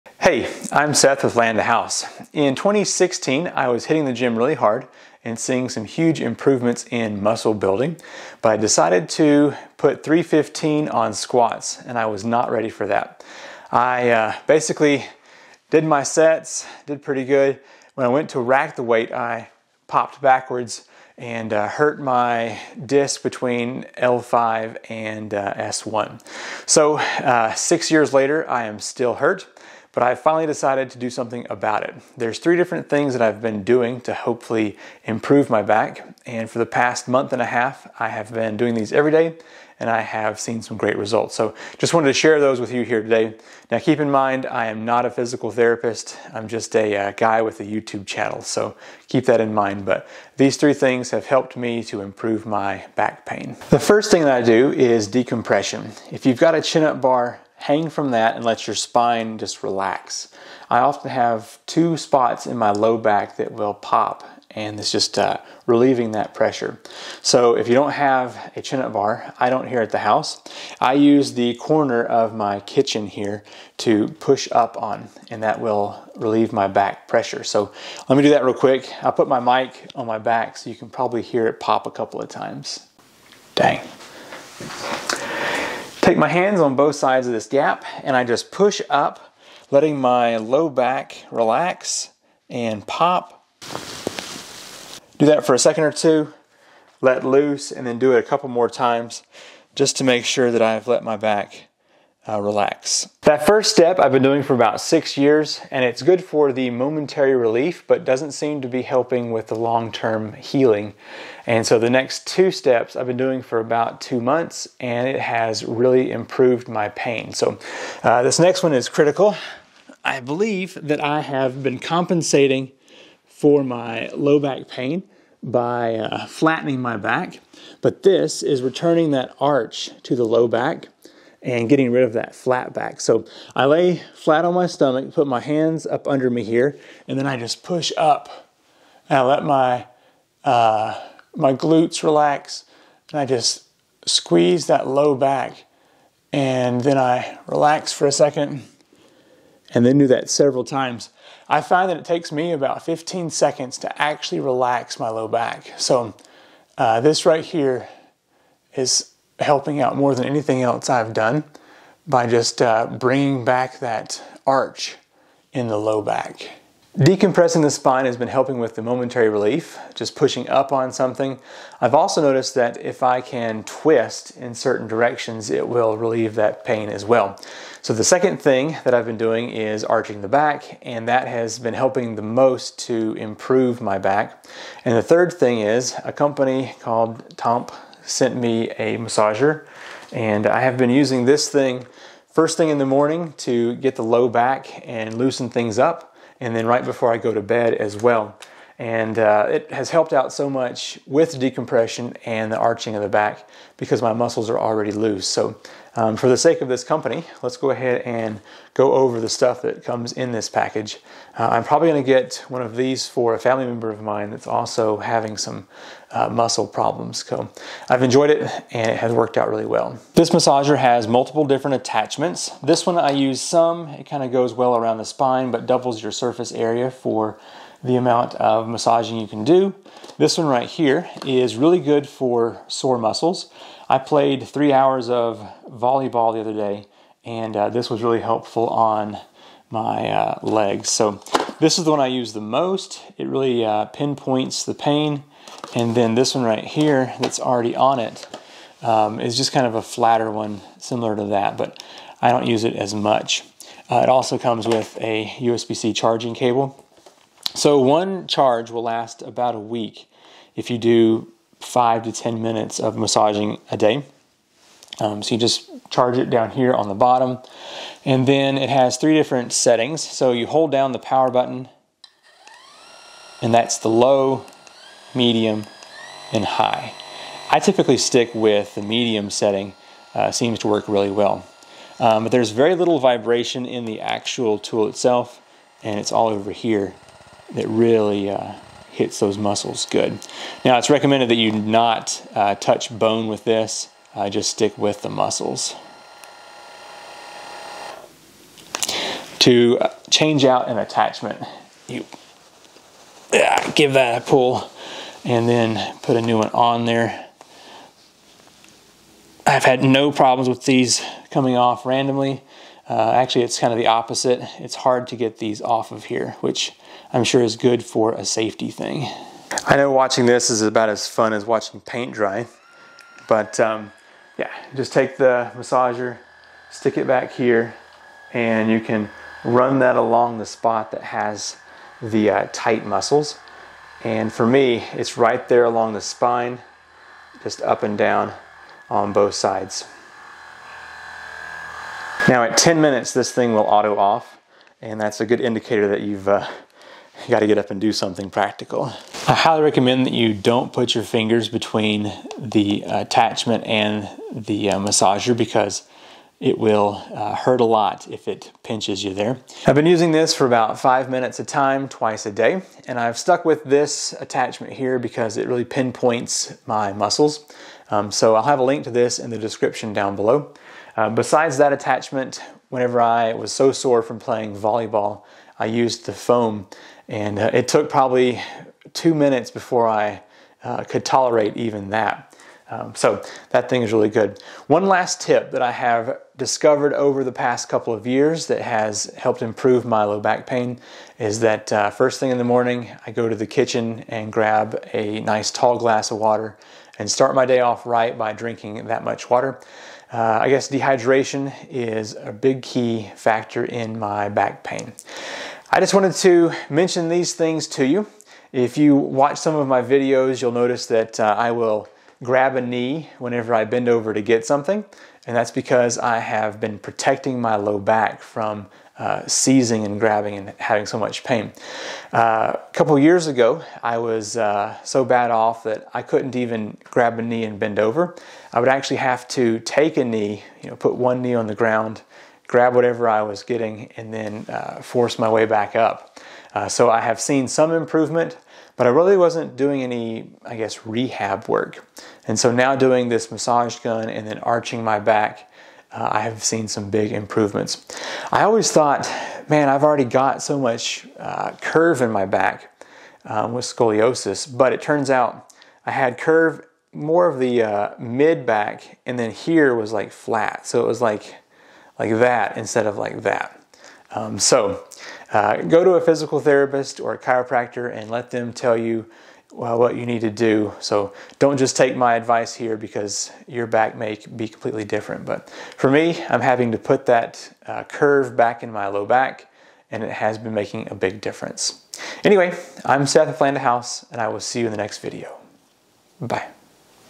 These 3 things are improving my low back health and reducing pain
Back in 2016 I hurt my low back trying to squat more than I was ready for. There are three things that have helped me to heal and relive the pain in my back. The first thing that I do when I wake up is use the massager along the spine to loosen up my back muscles. Next I lift myself up on a countertop to decompress my spine. Lastly I lay on my front and arch my lower back pushing up with my arms. These three have made a big difference over the past 2 to 3 months. A fourth tip is to drink plenty of water.